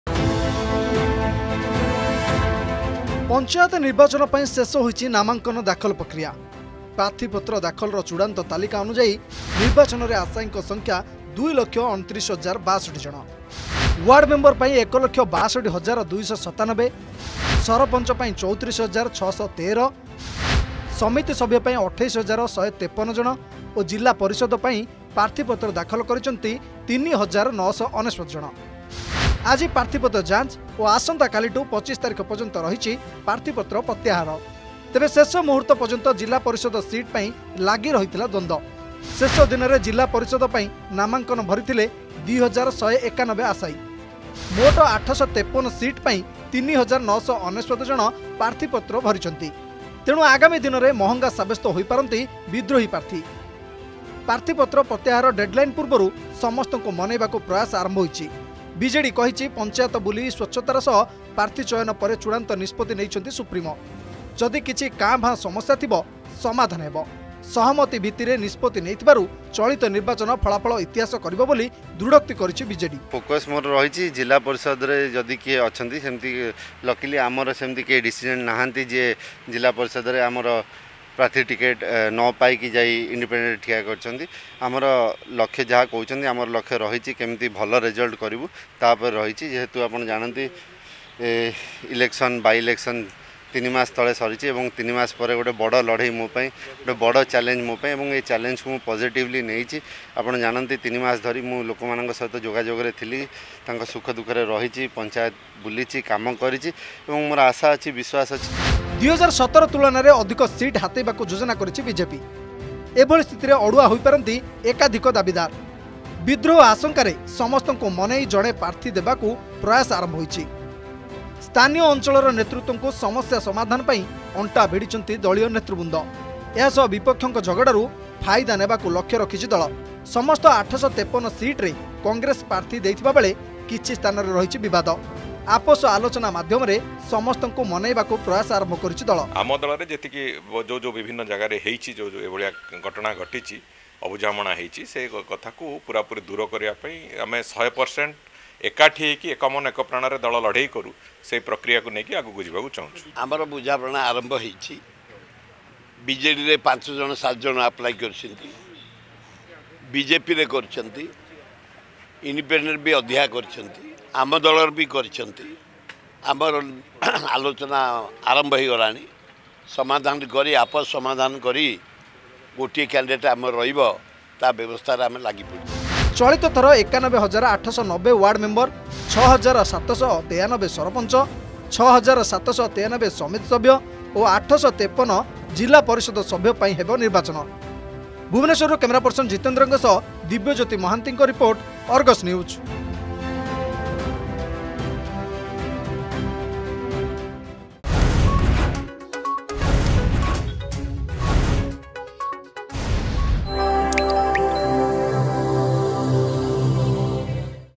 ପଞ୍ଚାୟତ ନିର୍ବାଚନ ପାଇଁ ଶେଷ ହୋଇଛି ନାମାଙ୍କନ ଦାଖଲ ପ୍ରକ୍ରିୟା
ପଞ୍ଚାୟତ ନିର୍ବାଚନ ପାଇଁ ଶେଷ ହୋଇଛି ନାମାଙ୍କନ ଦାଖଲ ପ୍ରକ୍ରିୟା । ପ୍ରାର୍ଥୀପତ୍ର ଦାଖଲର ଚୁଡାନ୍ତ ତାଲିକା ଅନୁଯାୟୀ ନିର୍ବାଚନରେ ଆଶାୟୀଙ୍କ ସଂଖ୍ୟା ୨ ଲକ୍ଷ ୨୯ ହଜାର ୬୨ । ୱାର୍ଡମେମ୍ବର ପାଇଁ ୧ ଲକ୍ଷ ୬୨ ହଜାର ୨୯୭,ସରପଞ୍ଚ-୩୪ ହଜାର ୬୧୩,ସମିତି ସଭ୍ୟ ୨୮ ହଜାର ୧୫୩ ଓ ଜିଲ୍ଲା ପରିଷଦ ପାଇଁ ପାର୍ଥିପତ୍ର ଦାଖଲ କରିଛନ୍ତି ୩ ହଜାର ୯୯୯।